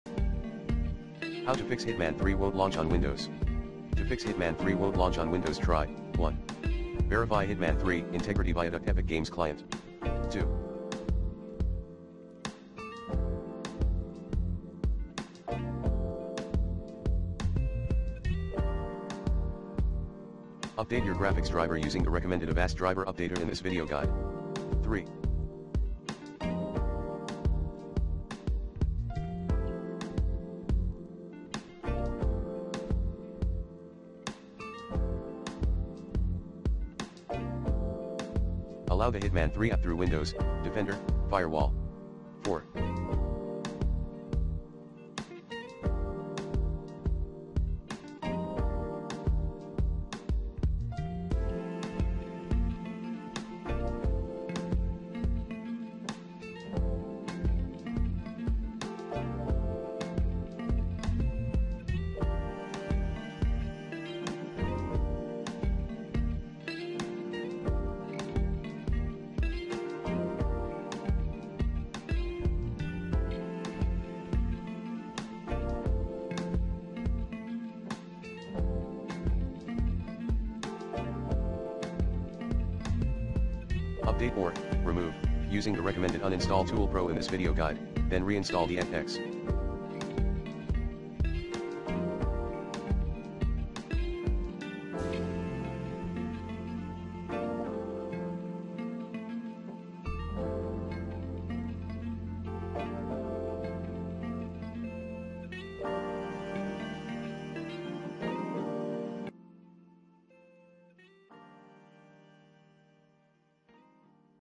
Fix Hitman 3 won’t launch on Windows 11/10
Here's how to Fix Hitman 3 won’t launch on Windows 11/10. Run Avast Driver Updater@ to keep your device drivers up-to-date. To effectively remove unwanted/problematic programs, run this best uninstall tool@